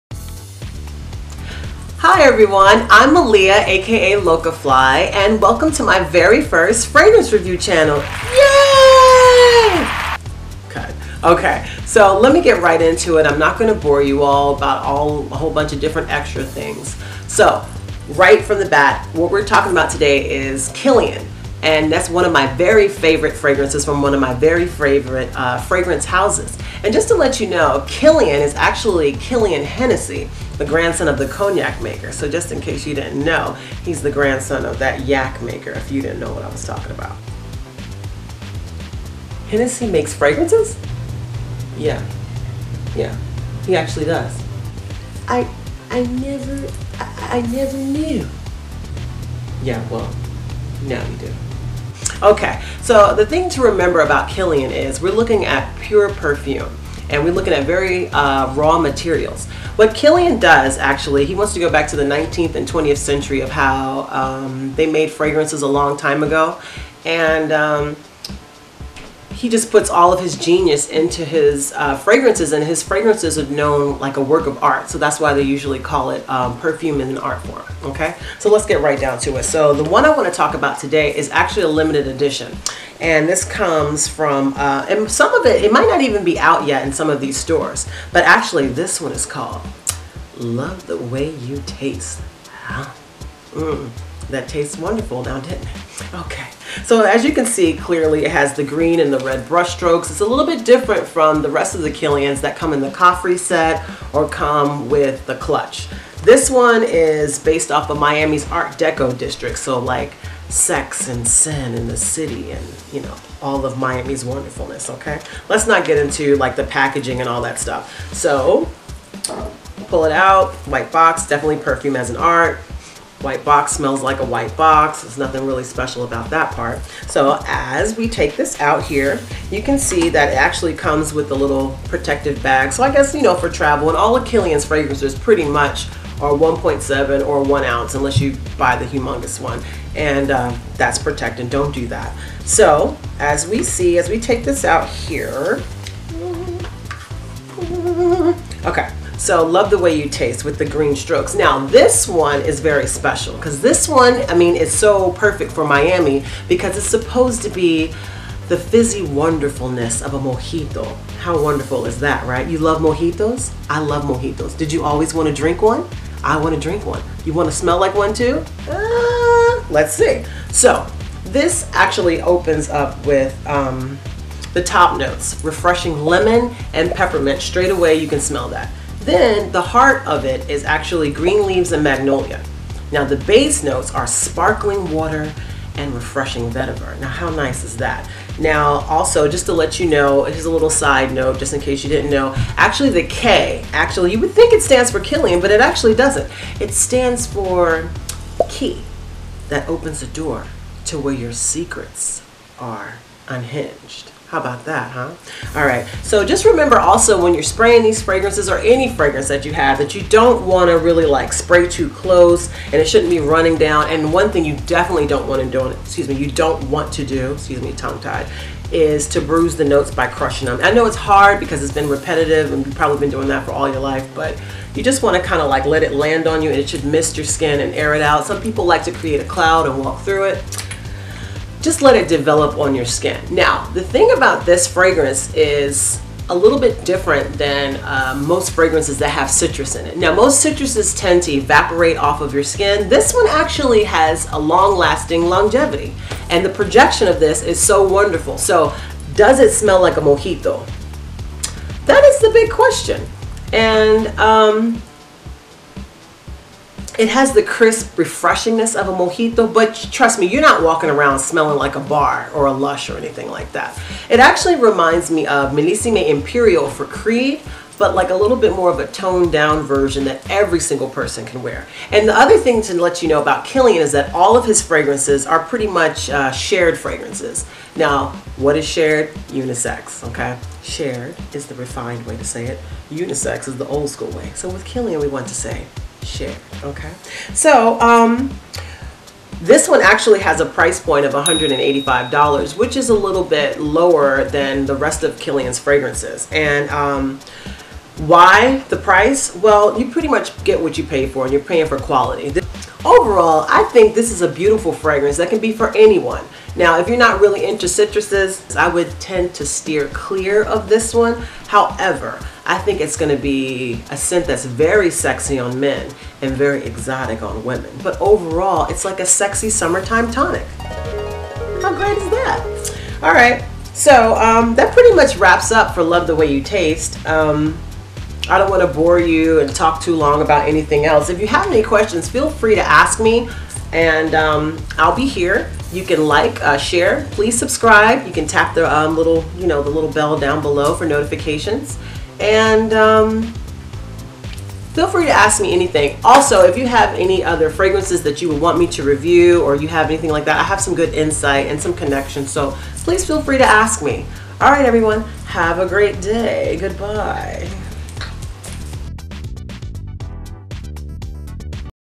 KILIAN- Love The Way You Taste review
(OK here’s something: in this video I did say that this fragrance is $185. In select Nordstrom and majority of Saks, it is $250. However, it is $185 in Bloomingdale’s. I don’t know for how long though,  seeing as how this is a limited edition fragrance.)

Okay, so not the original video, but here it is. This is a fragrance video review for Kilian's limited edition, Love The Way You Taste..
Shout out to @TonyBakerComedy for "I never..I never kneeew...😆
I love this fragrance!

top notes: lemon/peppermint
heart notes: green leaves/magnolia
LONG LASTING base notes: sparkling water/vetiver

reminiscent of a mojito. If any of you own this fragrance, or have  smelled it, or gained a sample, let me know what you think! (and i'm sure i forgot something...my bad)

shout out to IG: @mz_anamaria & @PremierEventMedia
(video footage & production)